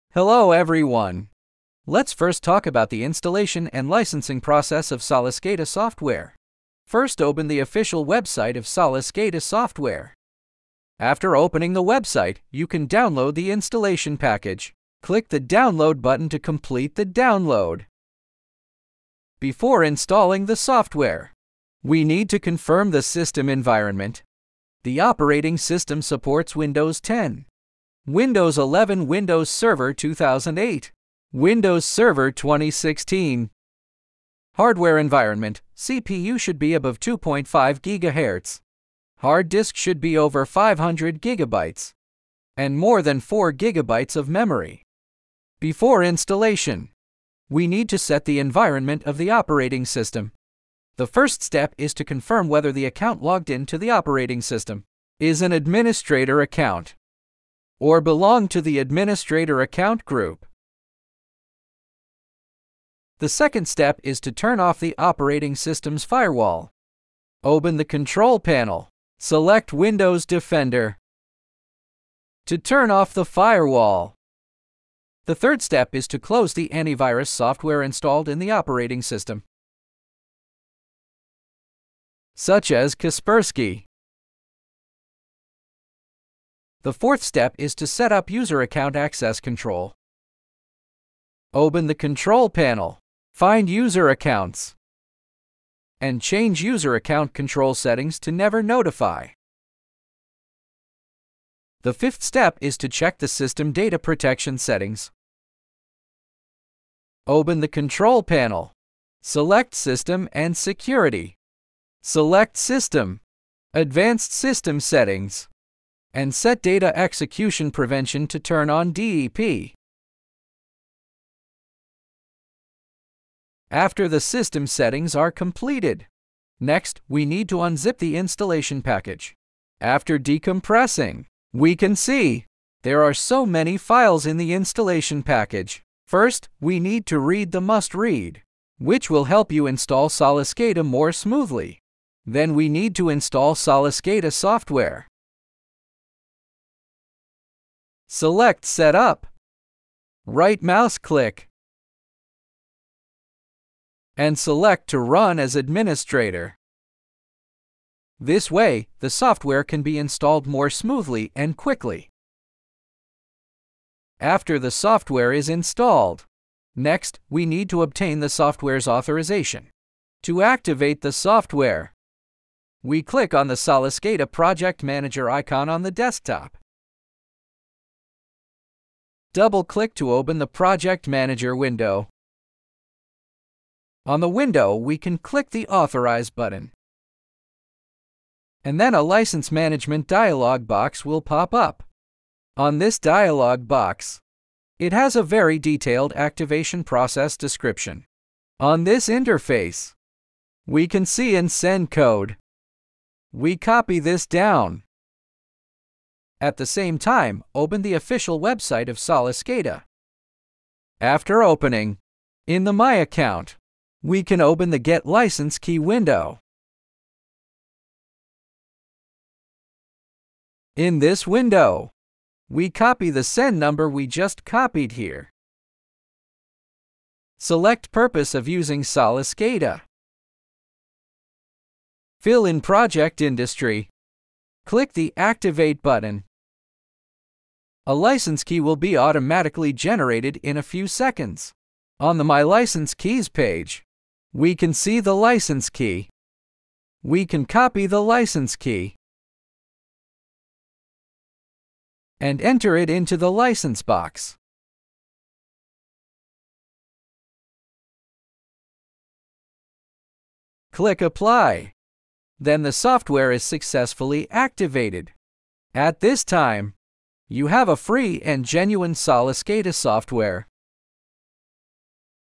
SOLISCADA Tutorials #1 - Installation and Licensing
This tutorial will show you the installation and licensing of our SOLISCADA.